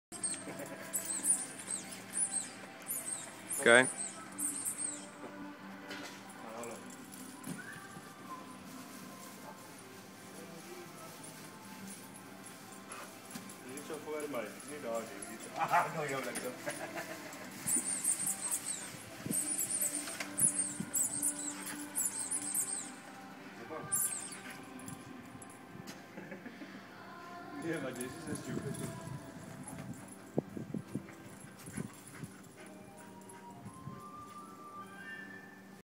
Rufus Playing in the Back Garden
Testing my new iPhone 4 HD video recorder in the back garden with my dog, Rufus.